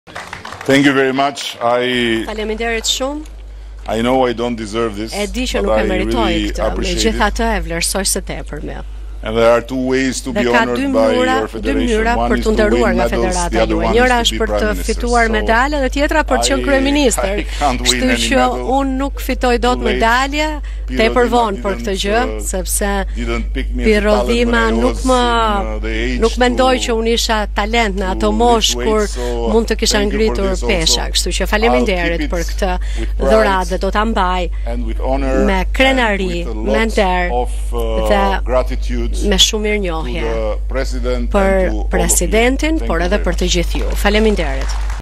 Report TV - Batuta e Ramës: Nuk do të merrja dot medalje nëse nuk do të isha kryeministër!
reportTV lajmeshqip lajme Kryeministri Edi Rama ka marrë një medalje mirënjohje për kontributin në fushën e peshëngritjes. Për këtë nuk kurseu batutat ndërsa tha se do e mbajë me krenari.

“E di që nuk e meritoj këtë por e pranoj. Ka 2 mënyra për tu nderuar nga ju, njëra për të fituar medalje, tjetra për të qenë kryeministër. Duke qenë se nuk fitoj dot medalje, tepër vonë për këtë gjë se Pirro Dhima nuk mendoi se mund të isha talent në atë moshë kur mund të kisha ngritur pesha. Këtë dhuratë do ta mbaj me krenari, nder dhe shumë mirënjohje për presidentin dhe të gjithë ju”, tha Rama.